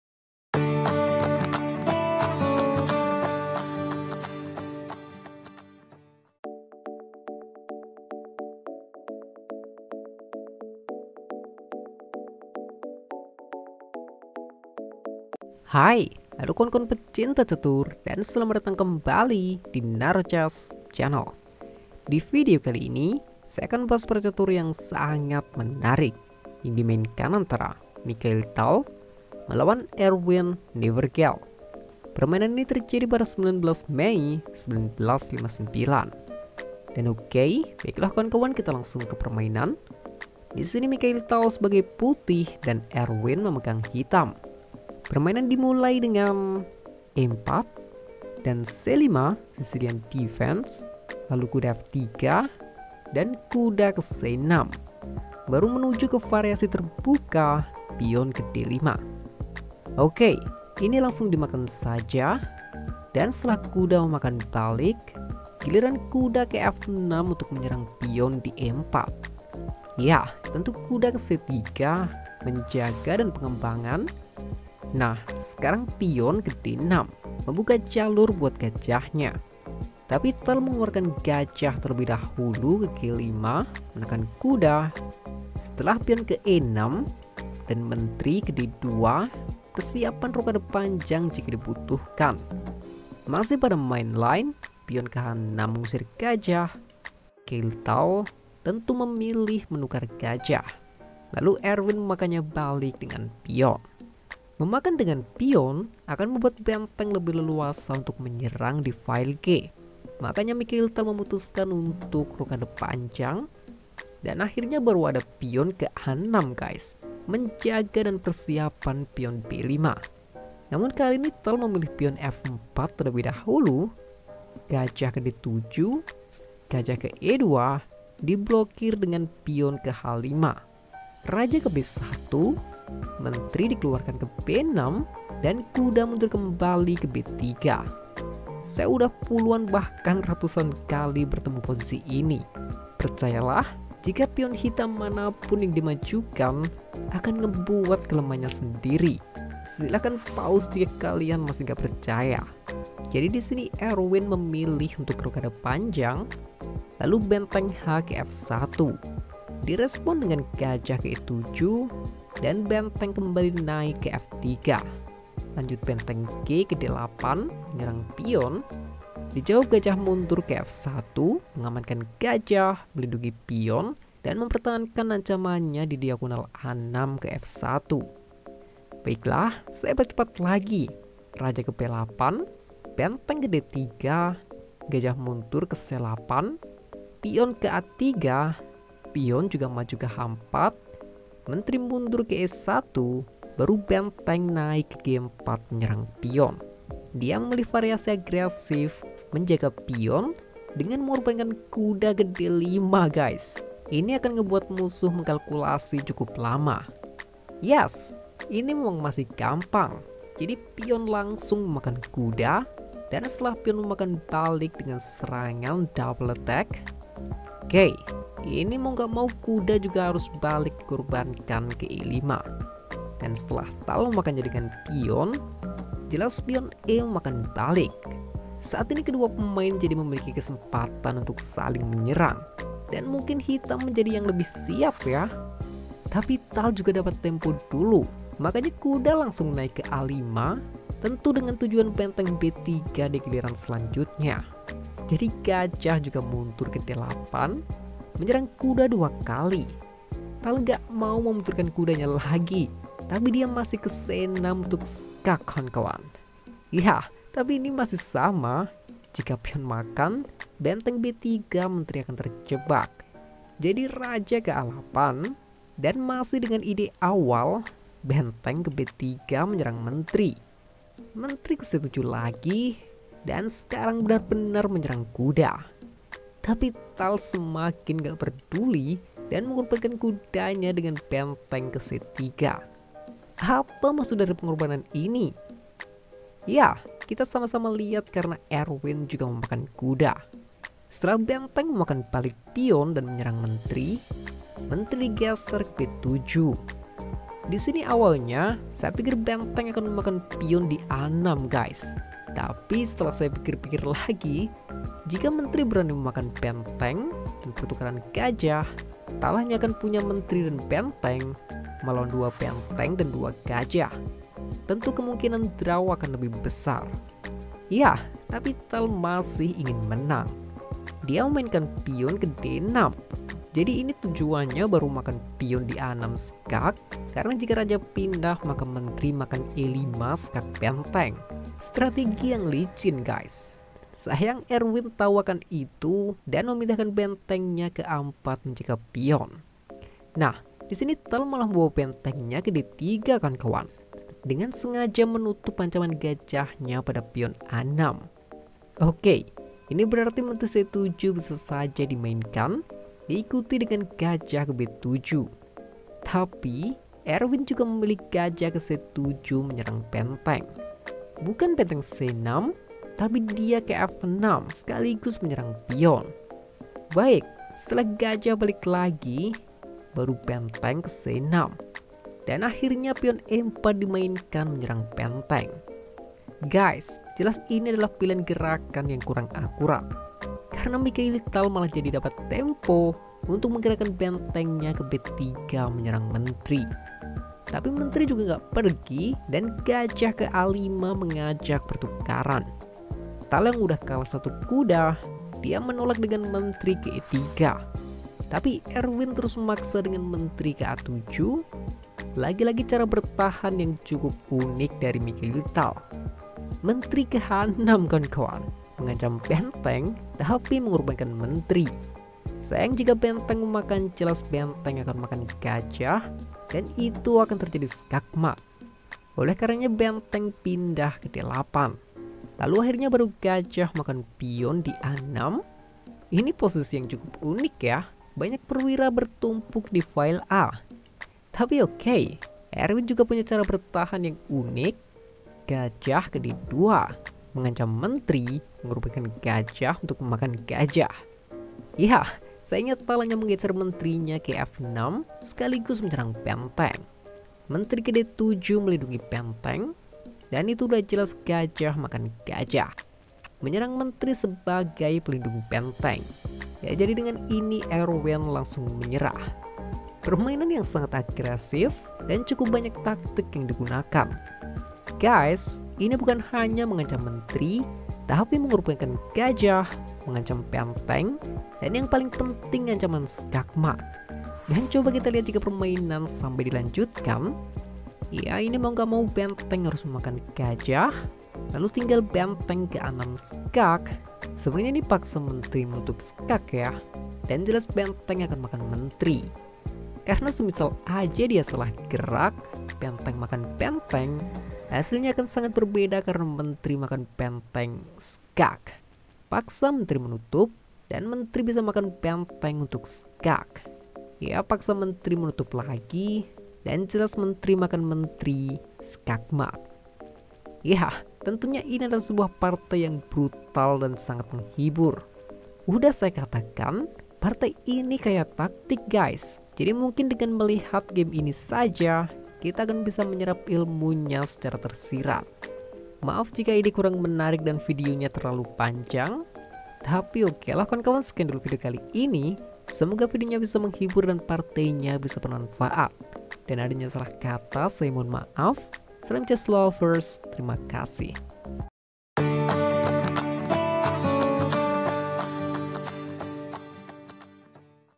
BELIAU MEMANG KAYA DENGAN TAKTIK !!!
haiii hallo kawan kawan pecinta catur dan selamat datang kembali di "Narro Chess" channel divideo kali ini saya membahas partai catur yang dimainkan antara
Mikhail Tal melawan Erwin Nievergelt  Permainan ini terjadi pada 19 mei 1959
UPLOAD SEADANYA DULU GUYS , EFFEK SAKIT GIGI WKWKWK

dan adanya salah kata saya mohon maaf ya 🙏
yang mau main chess.com kesini ya :)